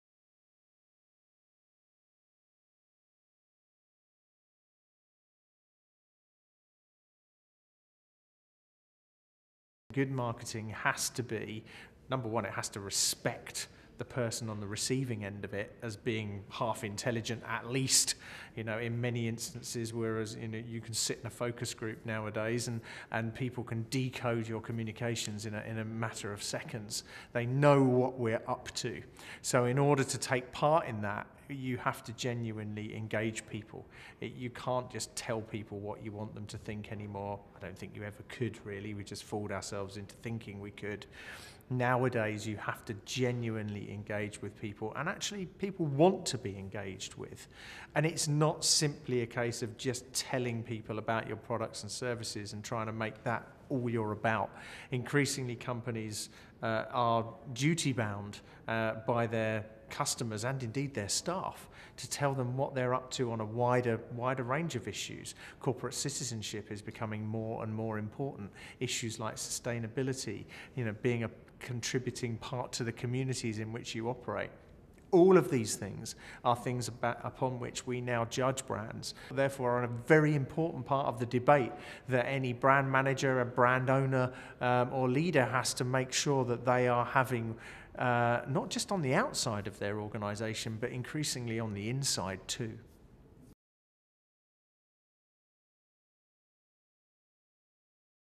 Full Spectrum Marketing
Good marketing goes beyond telling people about your products and services, explains Jez Frampton, Global CEO of Interbrand,  "Increasingly, companies are duty bound by their customers and indeed their staff to tell them what they're up to on a wider range of issues."